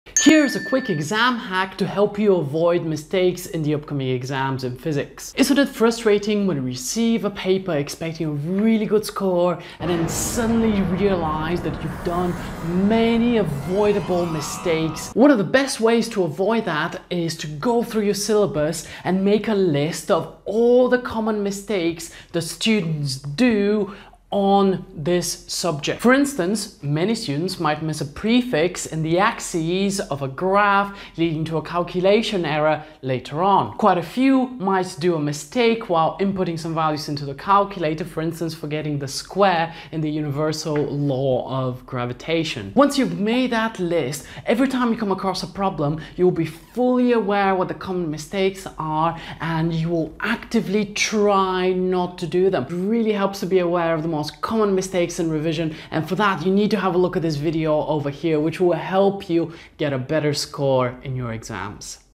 EXAM HACK to help you in Physics exams
How to study physics? How to learn physics?
 Exam skills in A Level Physics are tricky to achieve. The typical advice is to practice, my advice is to have thoughtful practice and make a list of all the common mistakes and be aware of all of them.
This tip is applicable to all exam boards including aqa a level physics, ocr a level physics, edexcel, cia etc. Hope this is helpful!